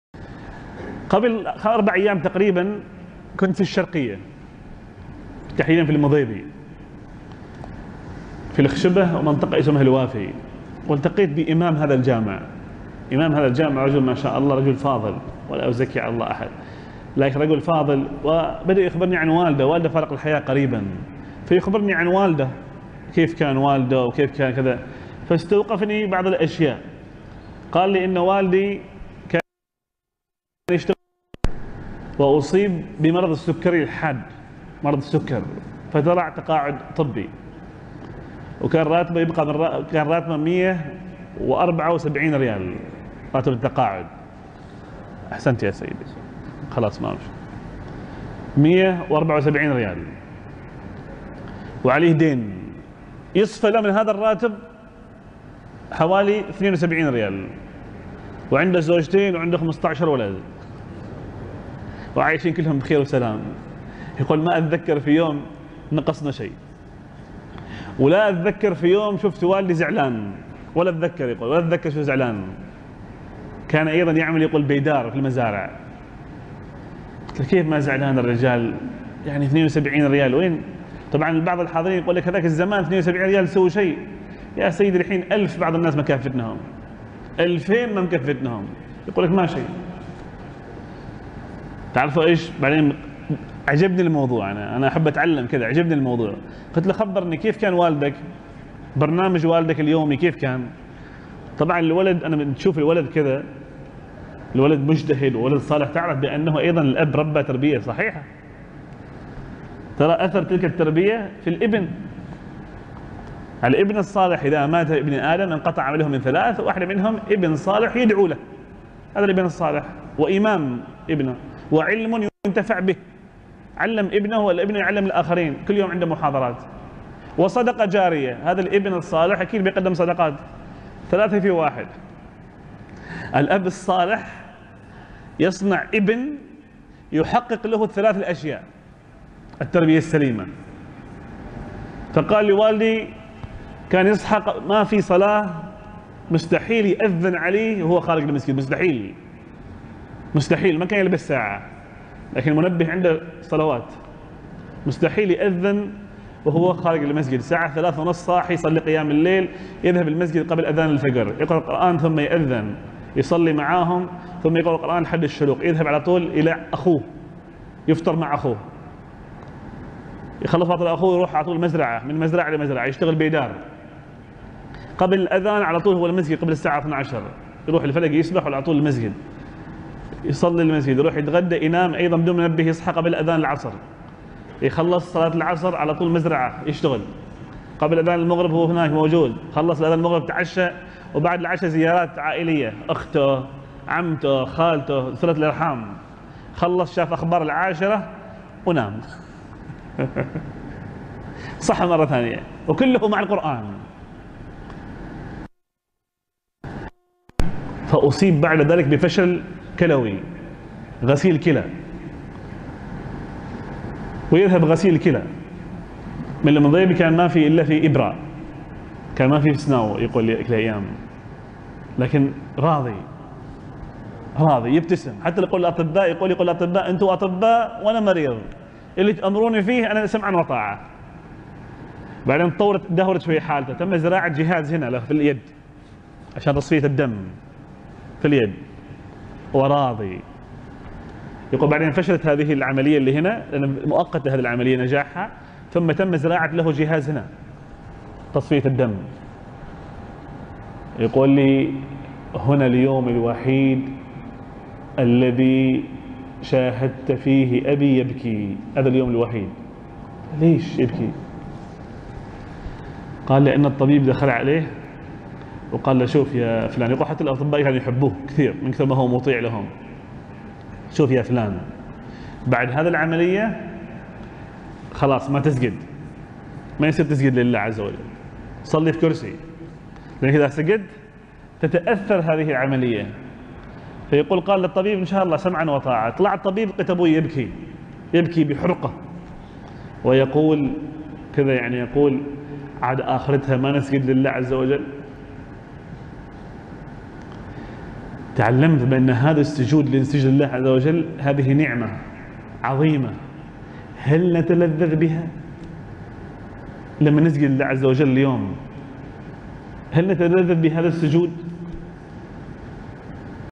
المدرب طلال الرواحي يُحدثنا عن رجل فاضل حسن الخصال عظيم السجايا
مصدر المقطع:

الجمعة 18رمضان 1440هـ محاضرة بجامع الريان بعنوان شي مما تعلمت

للاستاذ طلال الرواحي